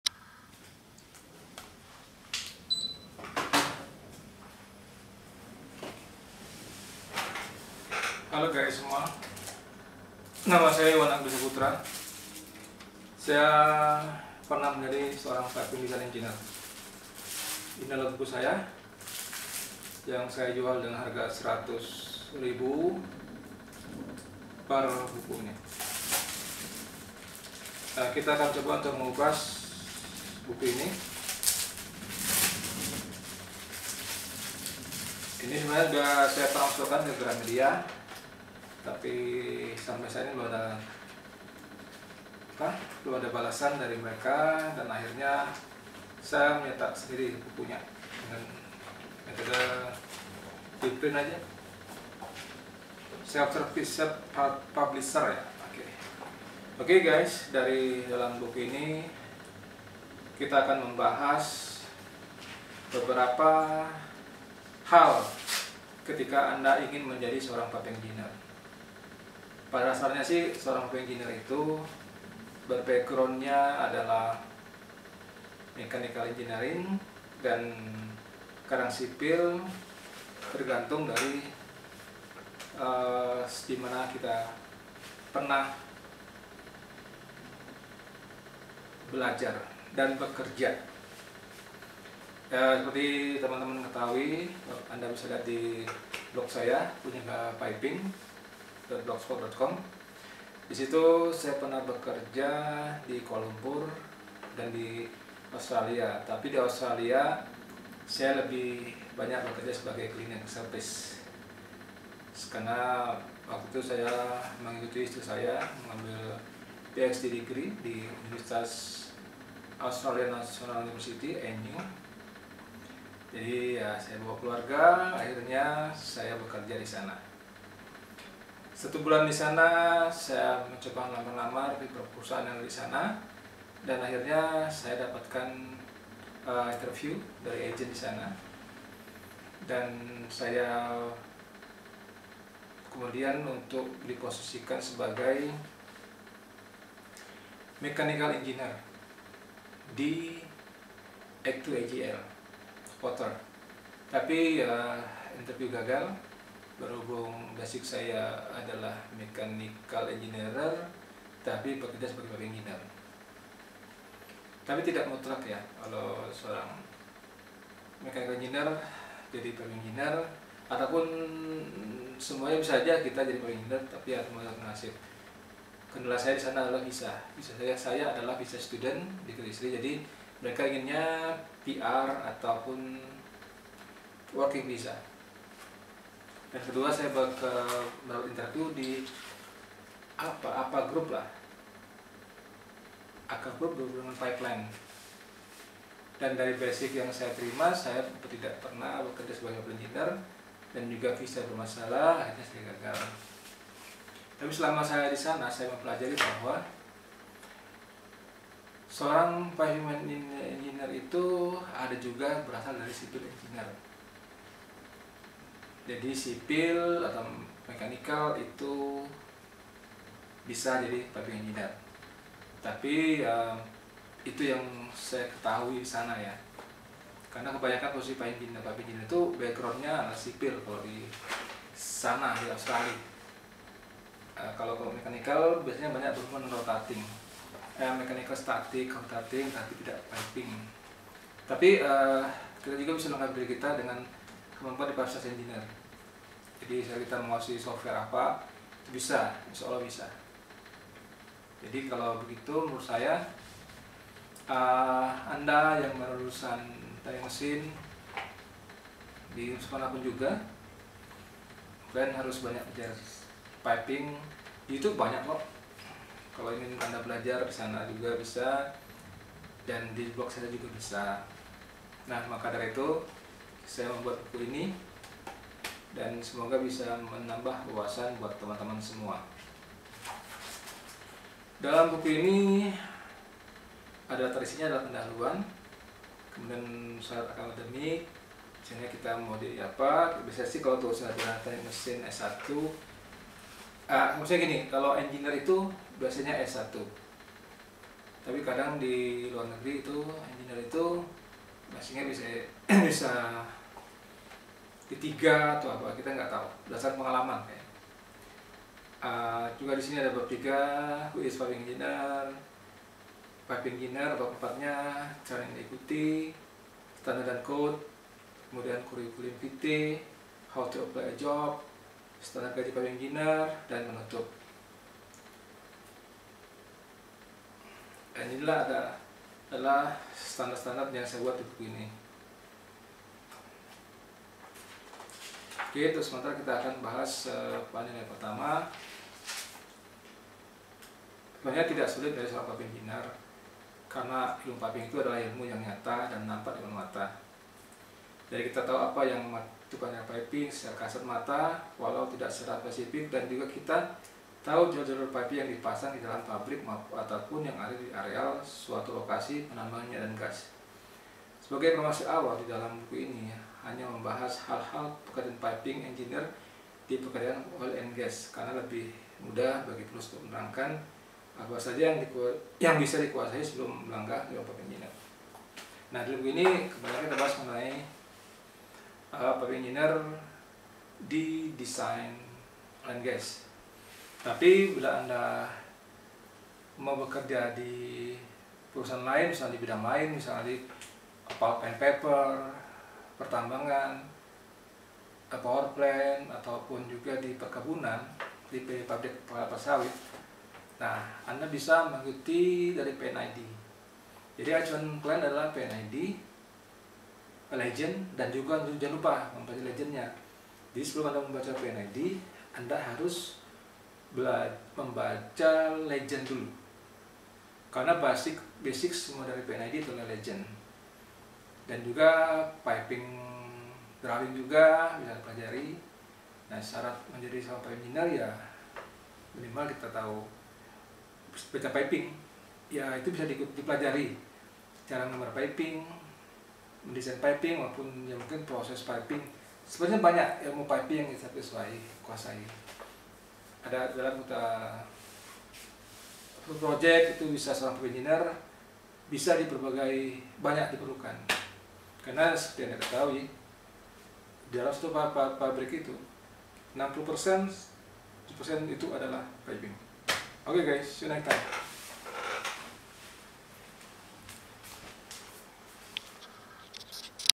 BUKU BILAMANA AKU INGIN MENJADI SEORANG PIPING ENGINEER
Sedikit Sharing mengenai buku Bila Aku Ingin menjadi seorang Piping Engineer . Dimana buku itu berisi pengalaman saya ketika menjadi seorang piping engineer.
Buku saya jual seharga 100 ribu / buah dan bisa anda beli di atau bisa juga comment di youtube ini